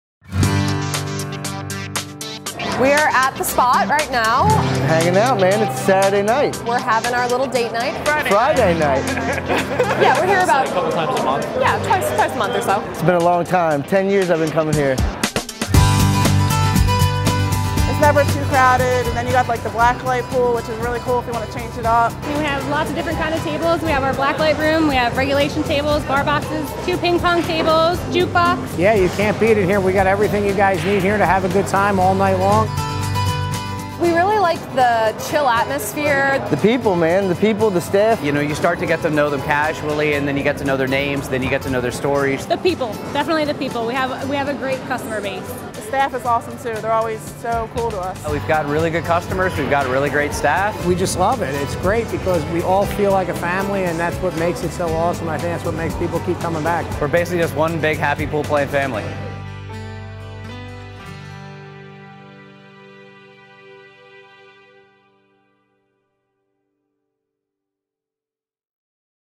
Fusco's The Spot | Promotional Video
Immerse yourself in the vibrant world of Fusco's The Spot with our exclusive promotional video! Nestled in the heart of the Northeast, "Fusco's The Spot" is a haven for fun and excitement. Join us on a thrilling journey through their fantastic facility, where great folks and unforgettable experiences come together. Get ready to witness the joy and excitement that awaits you – don't miss out on the fun-filled adventure!

Zimm Productions
ZimmProductions.com